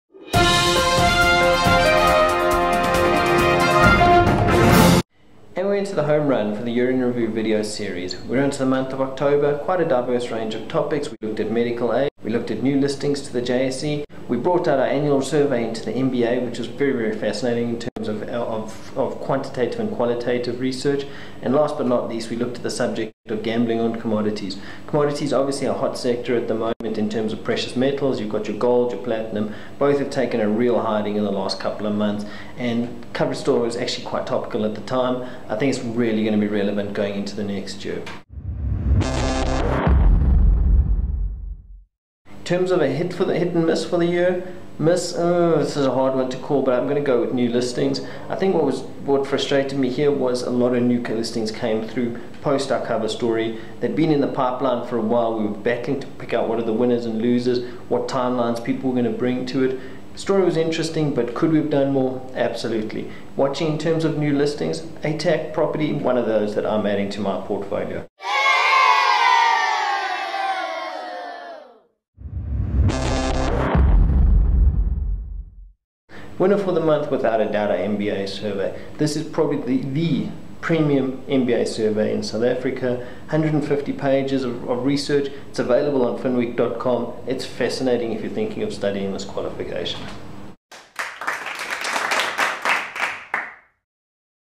Finweek's Year in Review: October 2013
The gift of hindsight is nothing to be scoffed at. In this video series, Finweek editor Marc Ashton looks at all the 2013 Finweek cover stories. He looks at the stories that hit the mark, and admits to those that didn't quite make the cut. This is the October episode of 2013 in review.